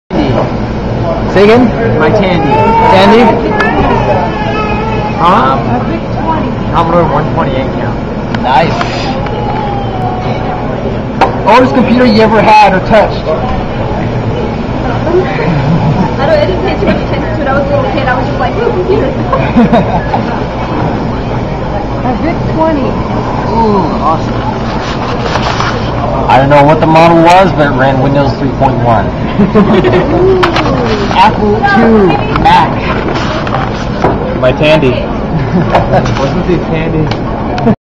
Animania 08 @ sunrise mall- after dinner!
yea we went around the table and asked what was the oldest computer u ever touched or had.